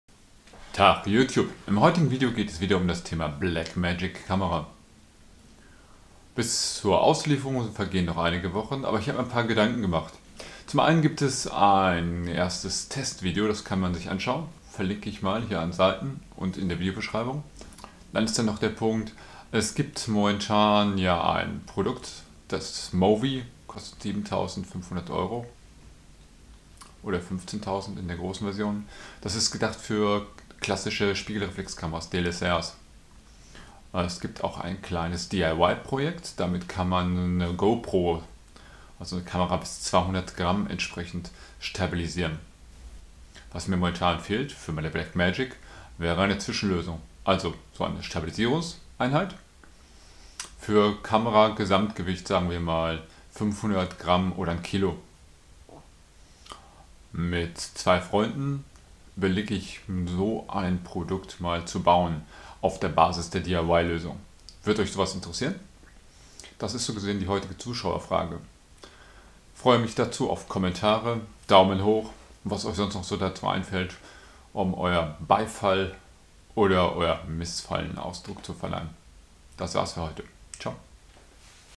Blackmagic Pocket DIY-Gimbal - Umfrage
Überlege mir mit zwei Freunden eine Stabilisierungslösung für die Blackmagic Pocket Camera zu bauen.